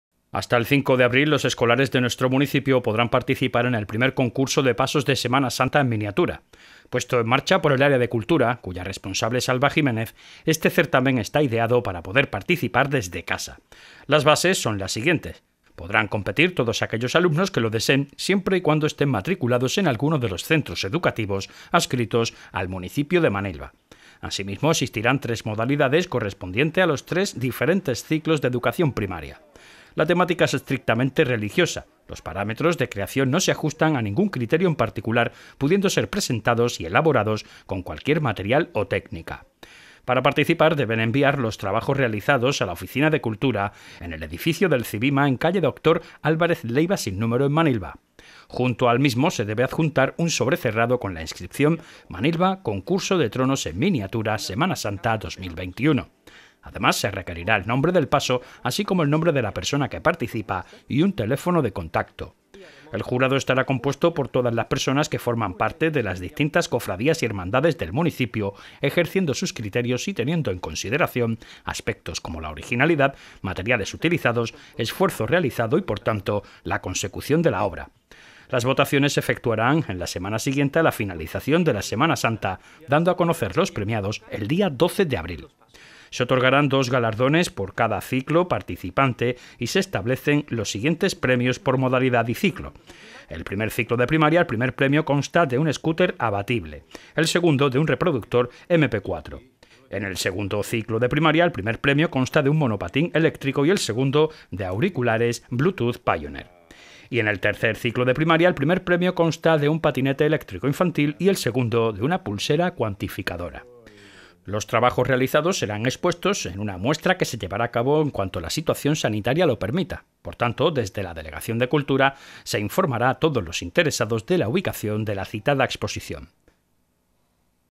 Sigue abierto el Concurso ‘Pasos Semana Santa en Miniatura’
Hasta el 5 de abril, los escolares de nuestro municipio podrán participar en el I Concurso de ‘Pasos de Semana Santa en Miniatura’.

Puesto en marcha por el área de Cultura, cuya responsable es Alba Jiménez, este certamen está ideado para poder participar desde casa.
Las bases del concurso son las siguientes:
Podrán competir todos aquellos alumnos que lo deseen, siempre y cuando estén matriculados en alguno de los centros educativos, adscritos al municipio de Manilva.
Asimismo, existirán tres modalidades, correspondientes a los tres diferentes ciclos de Educación Primaria. La temática es estrictamente religiosa (paso de Semana Santa en miniatura). Los parámetros de creación no se ajustan a ningún criterio en particular, pudiendo ser presentados y elaborados con cualquier material o técnica.
Para participar deben enviar los trabajos realizados a la oficina de Cultura, situada en el Edificio del Civima en Calle Doctor Álvarez Leiva, s/n en Manilva. Junto al mismo se debe adjuntar un sobre cerrado con la inscripción: “MANILVA CONCURSO DE TRONOS EN MINIATURA SEMANA SANTA 2021”; además se requerirá el nombre del paso, así como el nombre de la persona que participa y un teléfono de contacto.
El jurado estará compuesto por todas las personas que forman parte de las distintas Cofradías y Hermandades del municipio, ejerciendo sus criterios y teniendo en consideración aspectos como la originalidad, materiales utilizados, esfuerzo realizado y por tanto, la consecución de la obra.
Las votaciones se efectuarán en la semana siguiente a la finalización de la Semana Santa, dando conocer los premiados el día 12 de abril.
Se otorgarán dos galardones por cada ciclo participante y se establecen los siguientes premios por Modalidad/Ciclo: Primer Ciclo de primaria-Primer premio: Scooter Abatible 3 ruedas para niños. -Segundo premio: Reproductor MP4; Segundo Ciclo de primaria -Primer premio: Monopatín eléctrico -Segundo premio: Auriculares Bluetooth Pioneer; Tercer Ciclo de primaria -Primer premio: Patinete eléctrico infantil -Segundo premio: Pulsera cuantificadora Xiaomi.
Los trabajos realizados serán expuestos en una muestra que se llevará a cabo en cuanto la situación sanitaria lo permita. Por tanto, desde la Delegación de Cultura se informará a todos los interesados de la ubicación de la citada exposición.
La organización se reserva del derecho a añadir nuevas normas, cambiar total o parcialmente las normas o cualquier aspecto del concurso, de la misma forma que a adoptar nuevas iniciativas, que sin estar regulada en las presentes bases, contribuyan al mayor éxito y buen funcionamiento del concurso.